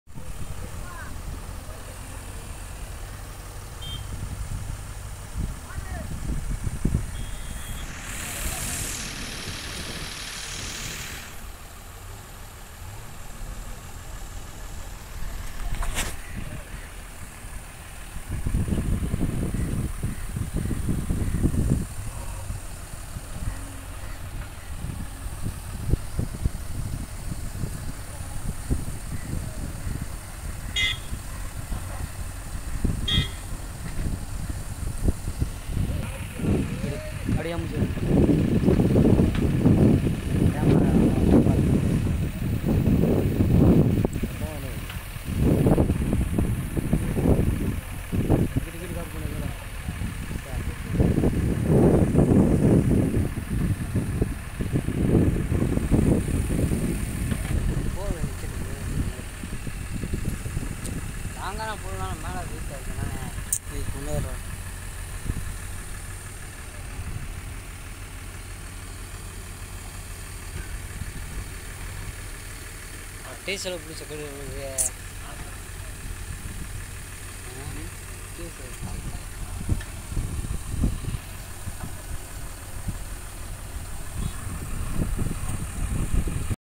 jallipatti
ஜல்லி பட்டி .சதுரகிரி முறுக்கு கம்பனிக்காக ஜாய்ஸ் போல் ஊன்றியது